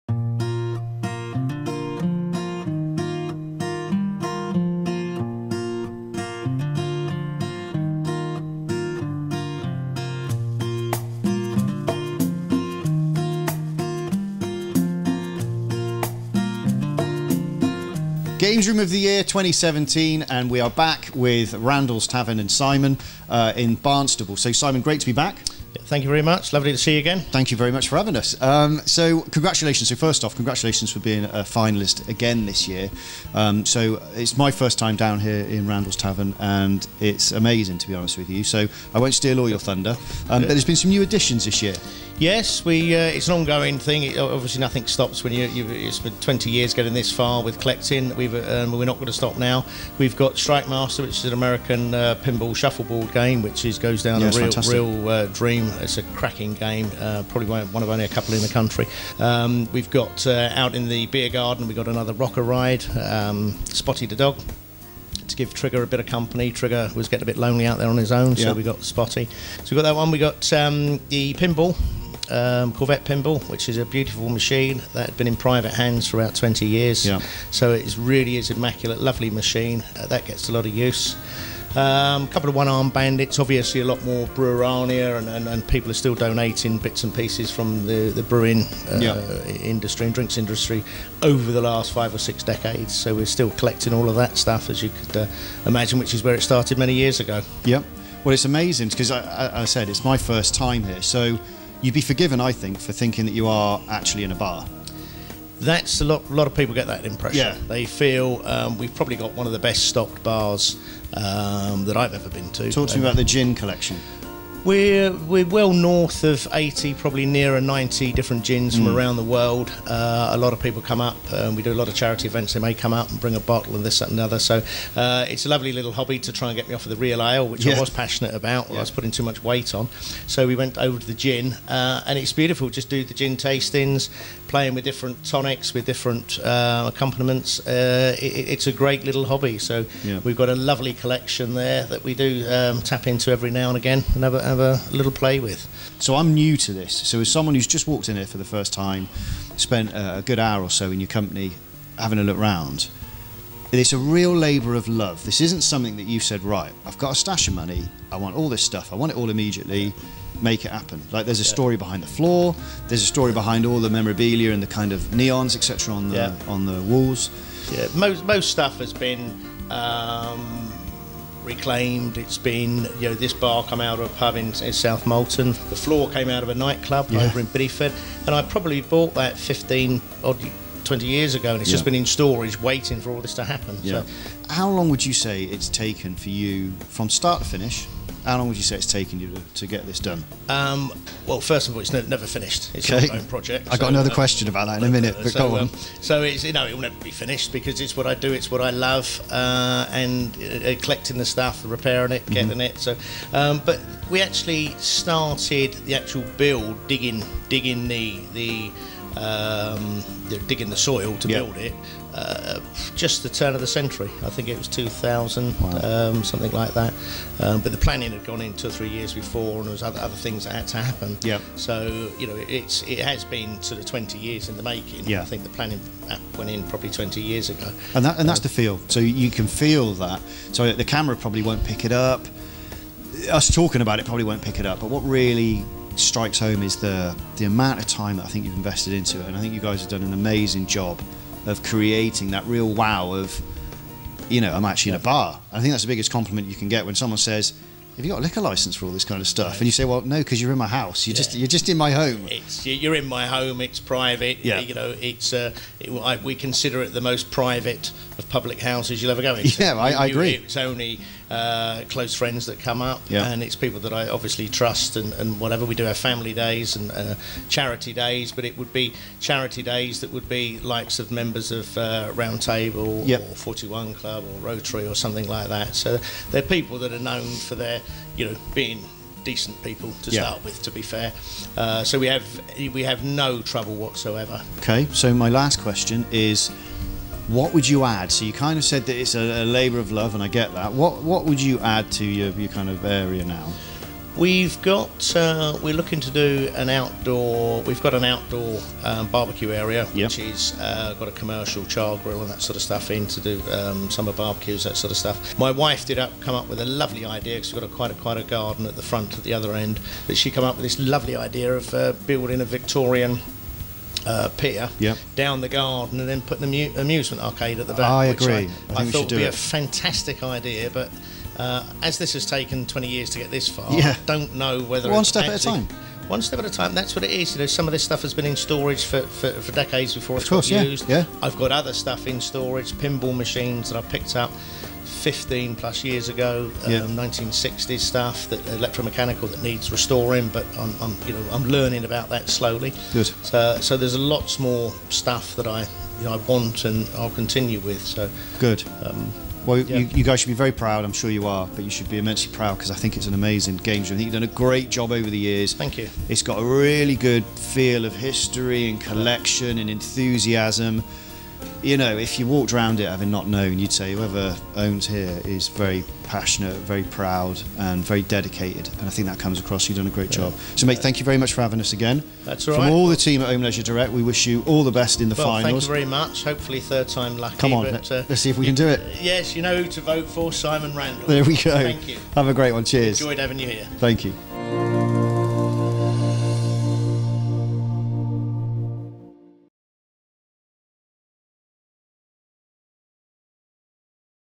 Games Room of the Year 2017 Finalists - Simon Randall
Games Room of the Year is back, and we decided to go and check out some of our brilliant finalists ourselves!

Simon Randall is in the finals again with his amazing home-built tavern. Randall’s Tavern brings together Simon’s love for old bar memorabilia and the ideal that pubs should be places for fun, as well as good drinks!

Vote for your favourite here!